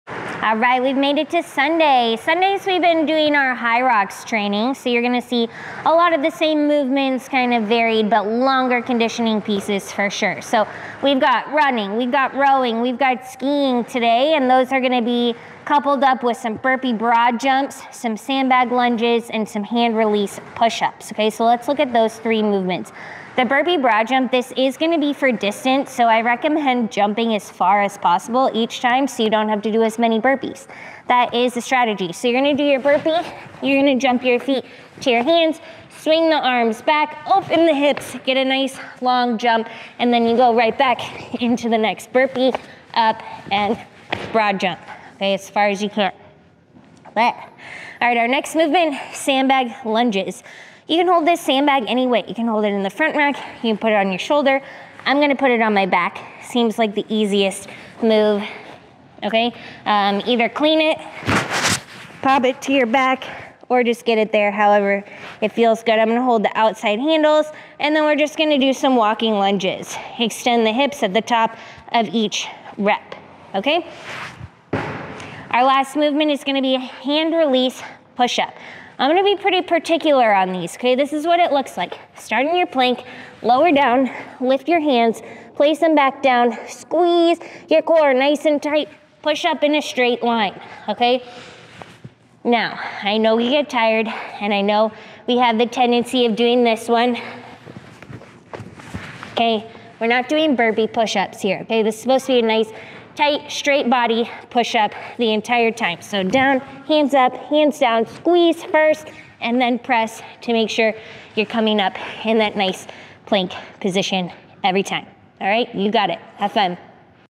Workout of the Day 10/13/2024
Metcon
Part 1:
Dynamic Warm Up
- high knees
- butt kicks
- lunge and rotate
- punter kicks
- lateral lunges
- inchworm with 2 push ups
- 200m run

Part 2:
AMRAP 40
1K run
30m burpee broad jumps
1K row
30m sandbag lunges
1K ski
30 hand release push ups

L1: 1/2 distance/reps, unweighted lunges
L2: 50/30 lb sandbag
Rx: 70/50 lb sandbag